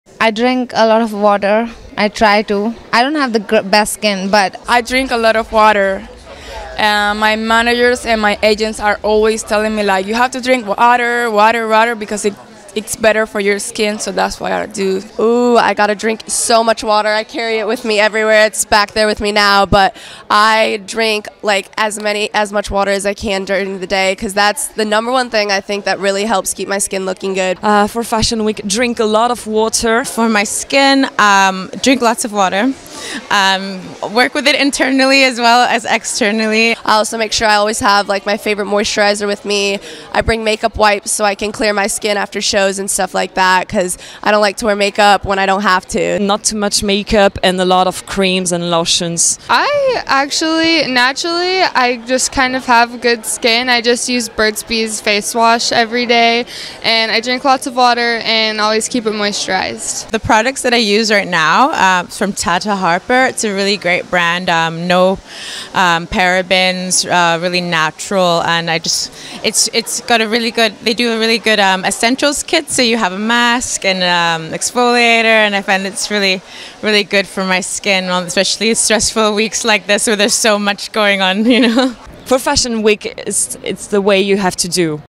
Model talks F/W 17-18 - Beauty tips - 6 | FashionTV
WORLD - FashionTV brings you an exclusive into what these flawless models do to keep their hair & skin beautiful. Featuring Radhika Nair, Richie Beras, Alanna Arrington, Astrid Pillette, Hannah Donke, and Emma Harris. Brought to you exclusively by FashionTV.